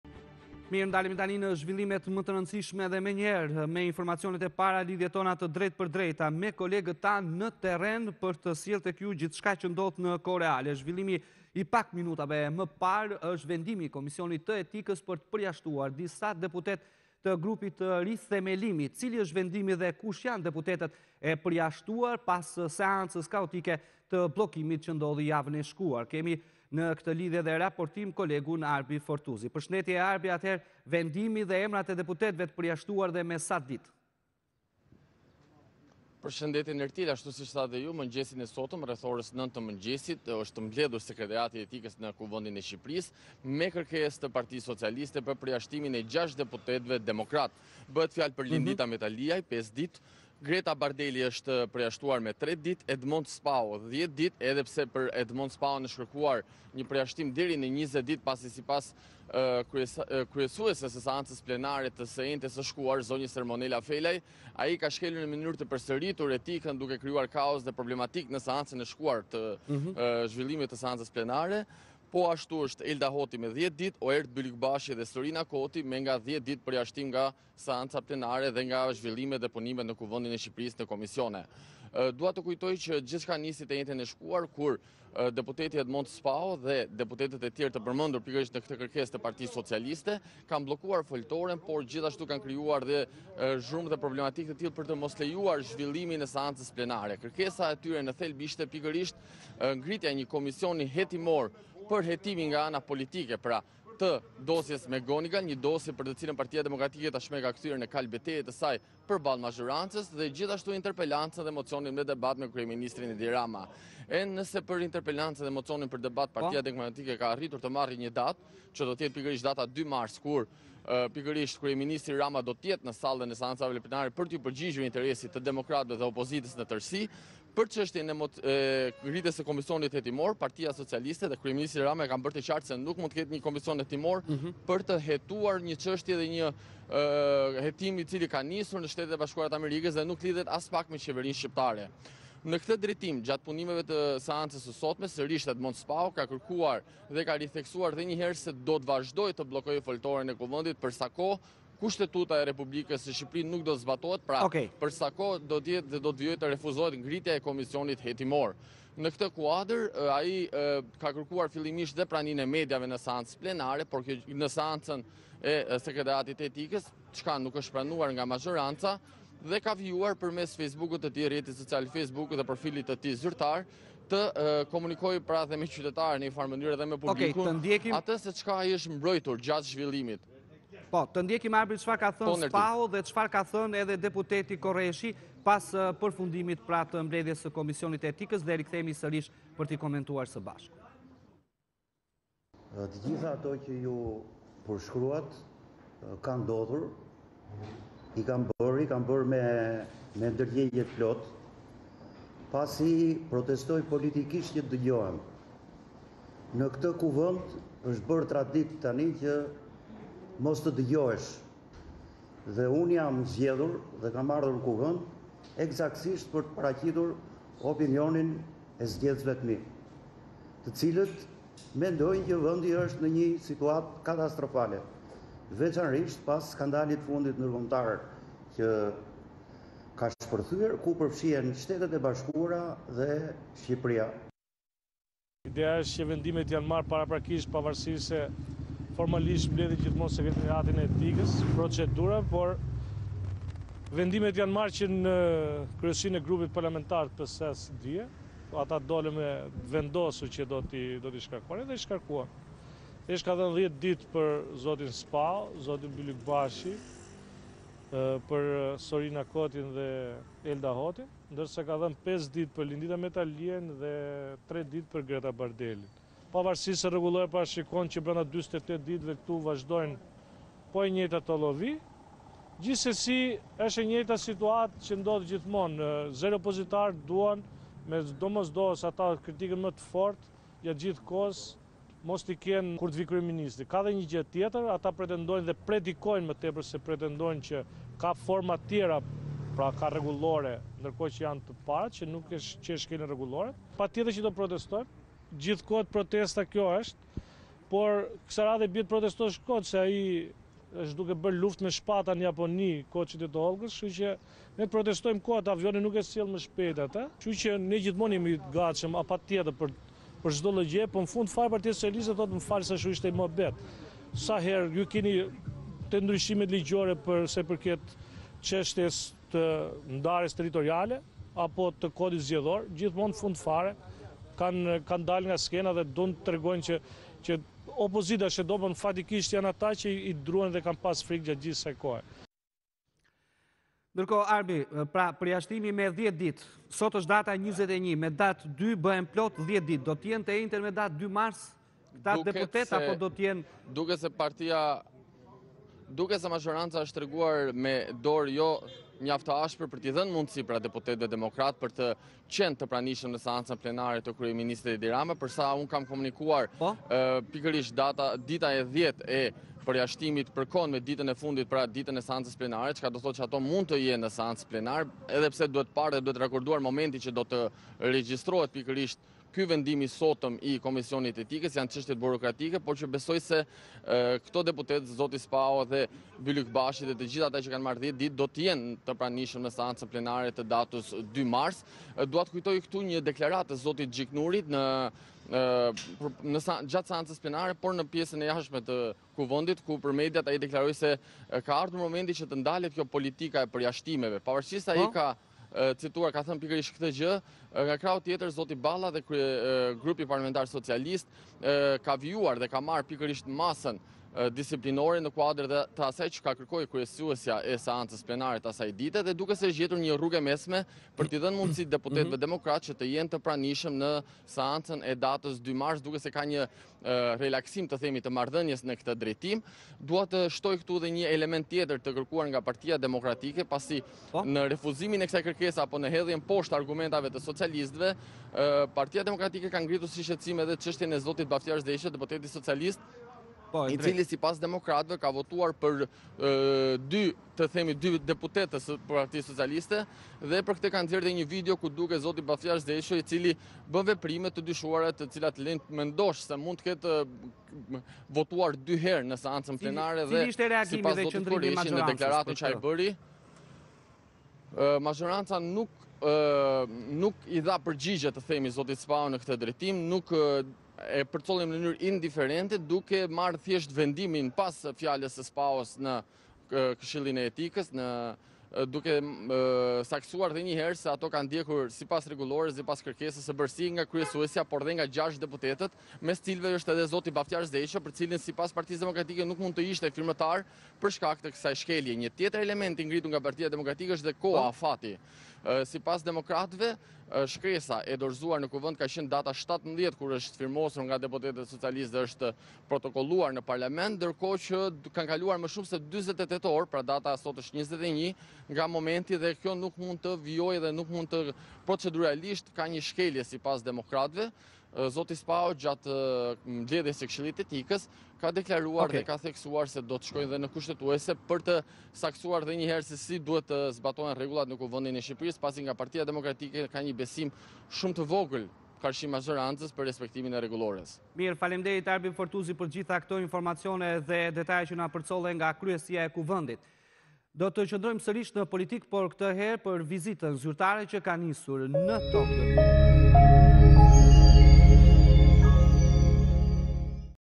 Bllokuan seancën, përjashtohen 6 deputetë të 'Rithemelimit'! 10 ditë për Spahon, deputeti: Do hyj
Sekretariati i Etikës ka vendosur sot përjashtimin e 6 deputetëve të PD-së, Edmond Spaho, Elda Hoti, Sorina Koti, Oerd Bylykbashi, Lindita Metaliaj dhe Greta Bardeli nga seancat plenare të Kuvendit për shkelje të etikës dhe bllokimin e foltores në parlament. Konkretisht Spaho, Bylykbashi, Koti dhe Hoti janë përjashtuar me 10 ditë nga seancat plenare, Metaliaj me 5 ditë dhe Bardeli me 3 ditë.